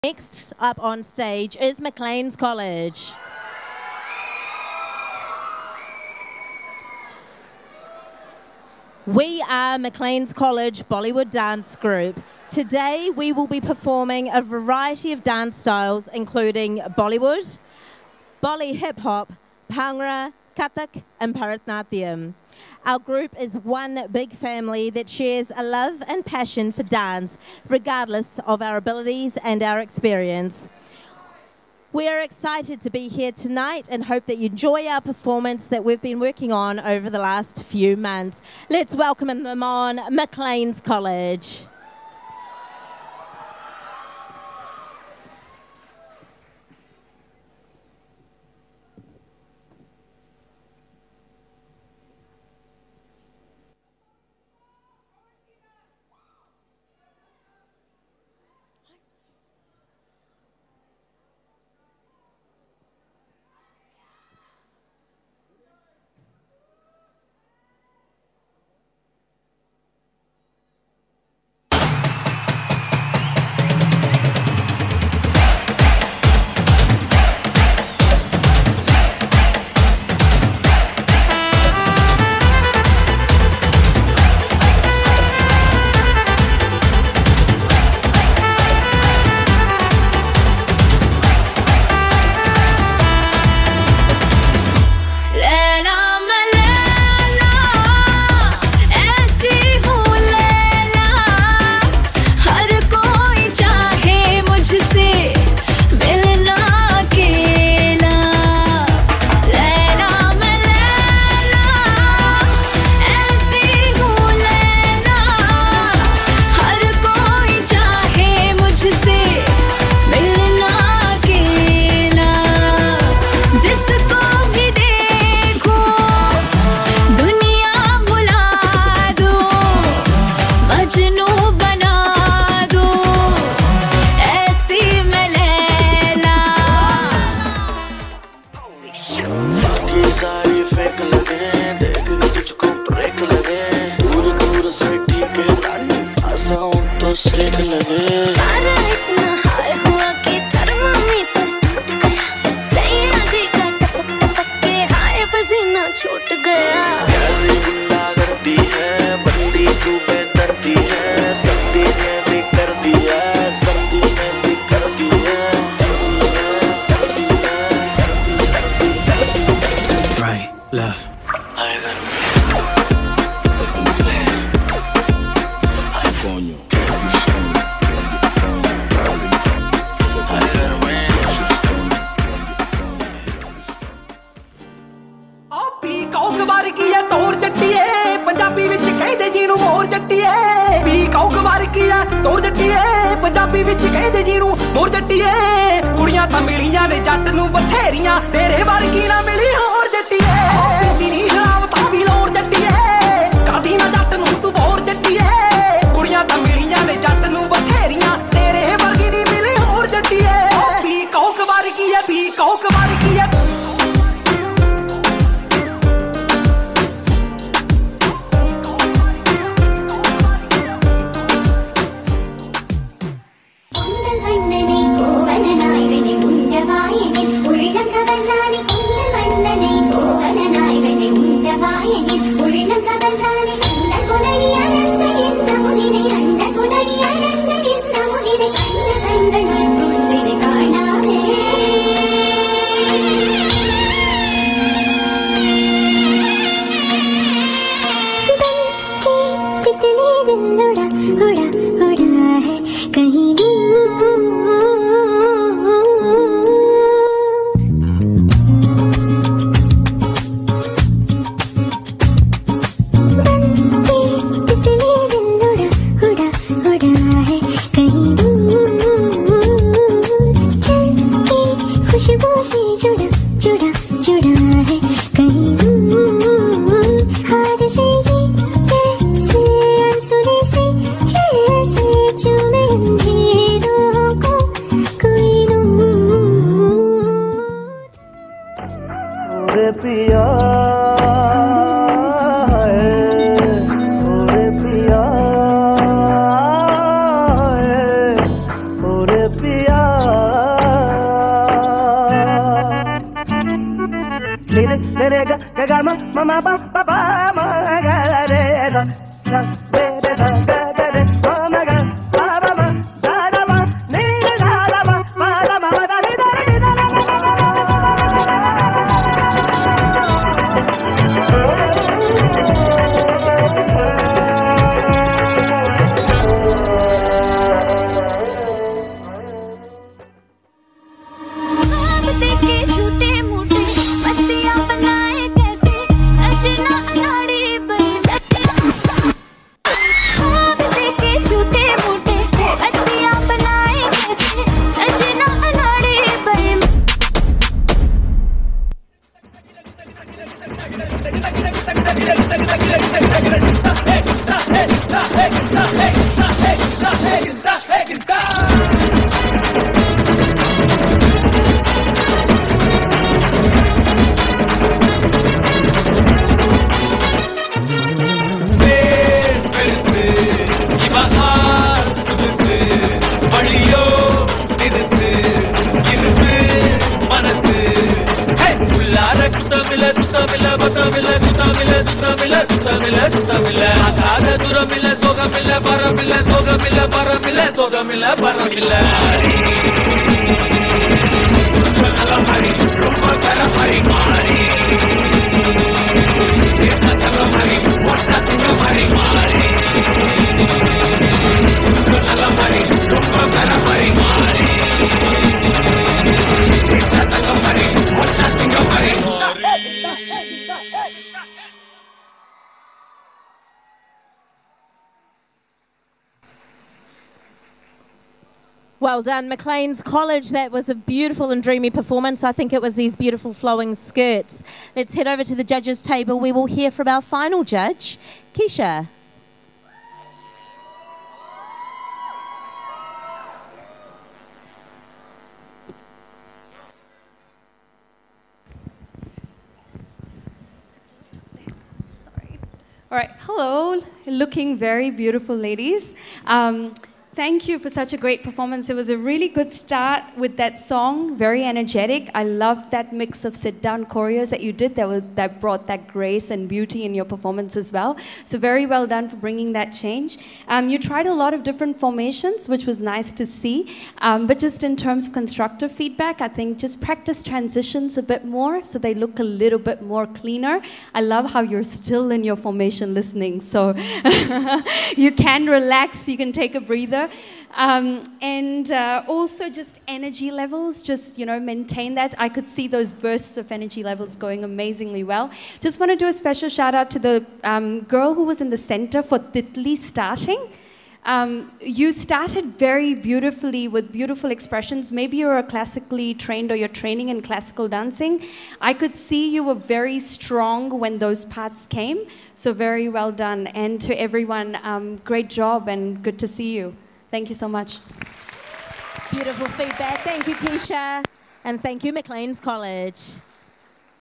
Macleans College - Bollywood Showdown 2023
We are the Macleans College Bollywood Dance Group. Today we will be performing a variety of dance styles including Bollywood, Bolly hip-hop, Bhangra, Kathak, and Bharatanatyam. Our group is one big family that shares a love and passion for dance regardless of our abilities and experience. We are very excited to be here tonight and hope you enjoy our performance that we have been working on over the past few months.